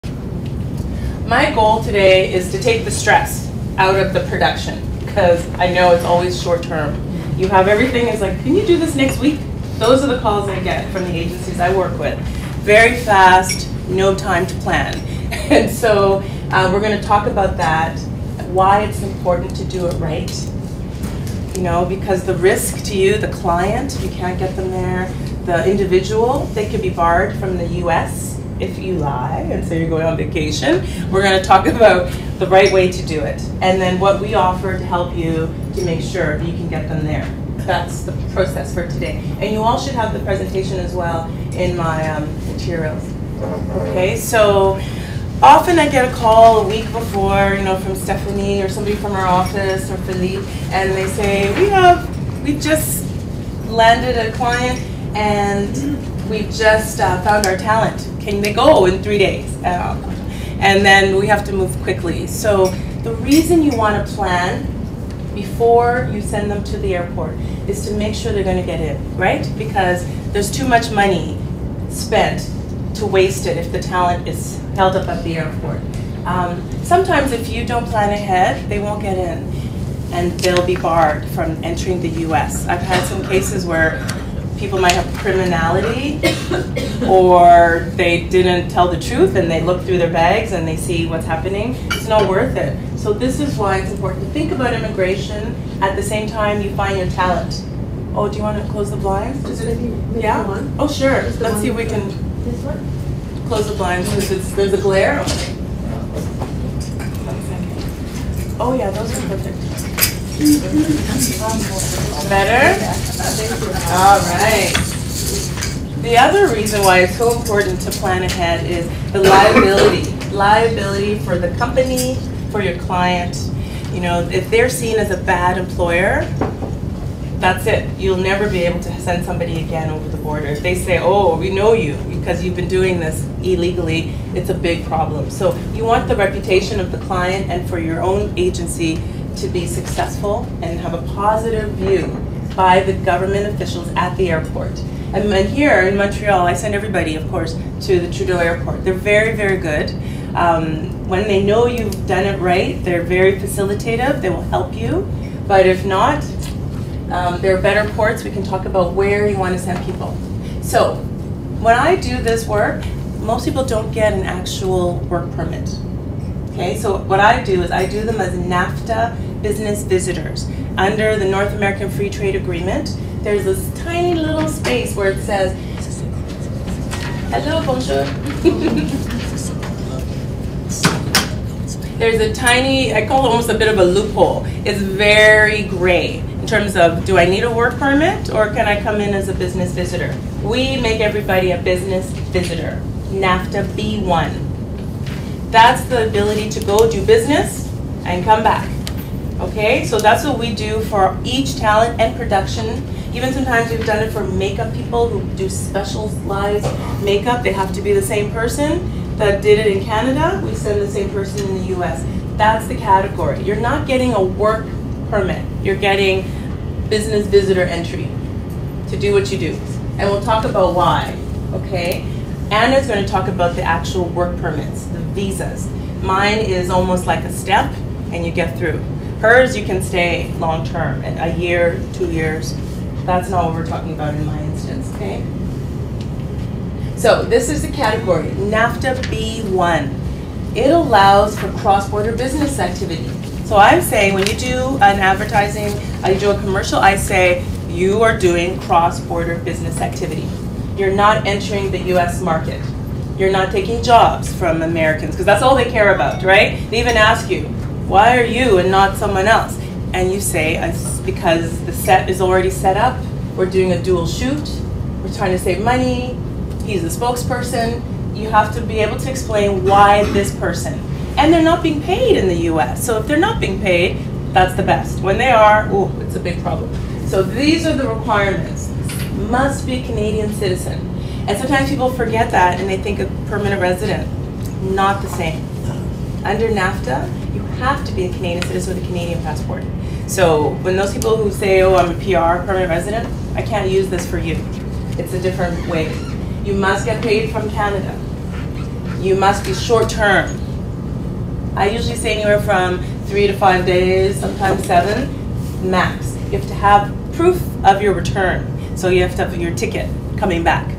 Canada Immigration Expert Evelyn Ackah Presents Crossing Borders Seamlessly
Evelyn Ackah’s presentation on Crossing Borders – Taking the Stress out of Shooting Commercials in the US, in Montreal, Quebec, April 8 2015.

𝑨𝒄𝒌𝒂𝒉 𝑩𝒖𝒔𝒊𝒏𝒆𝒔𝒔 𝑰𝒎𝒎𝒊𝒈𝒓𝒂𝒕𝒊𝒐𝒏 𝑳𝒂𝒘
𝑾𝒆 𝒅𝒐𝒏’𝒕 𝒔𝒆𝒍𝒍 𝒅𝒓𝒆𝒂𝒎𝒔, 𝒘𝒆 𝒔𝒆𝒍𝒍 𝒔𝒖𝒄𝒄𝒆𝒔𝒔.™

Our team has over 260+ five-star Google reviews! ⭐⭐⭐⭐⭐